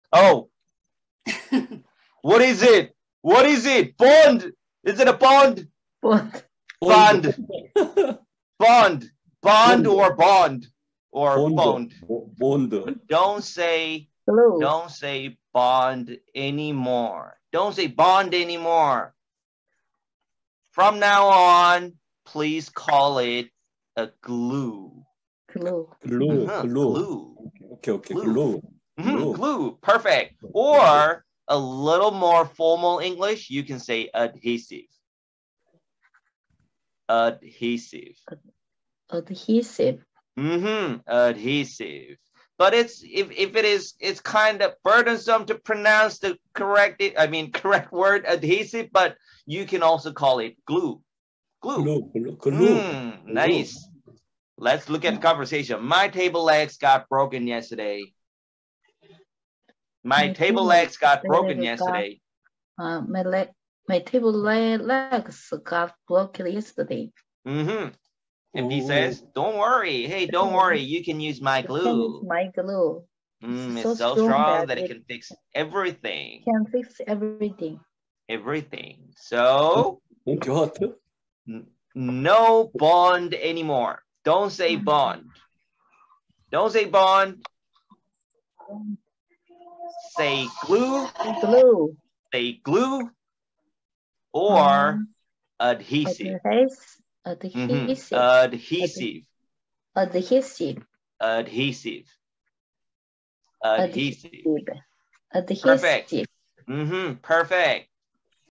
콩글리쉬 탈출하기 19-10
[KONGLISH vs ENGLISH]
bond  vs  glue
숭실사이버대학교 실용영어학과
# 구일민 교수님